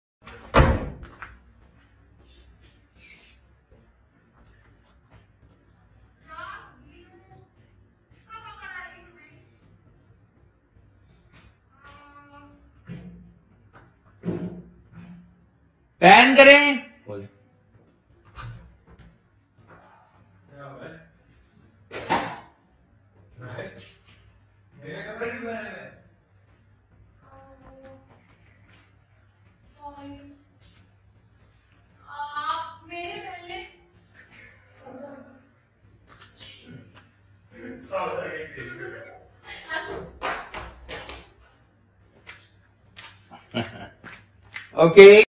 Chupke Chupke BTS With Ayeza Khan and Osama Khalid | Chupke Chupke Behind the Scene | Pak Showbiz PK
In This Video, You will see Ayeza Khan and Osama Khalid Behind the Scene BTS in Chupke Chupke. Hope You will Enjoying.

We have legal right to upload this video.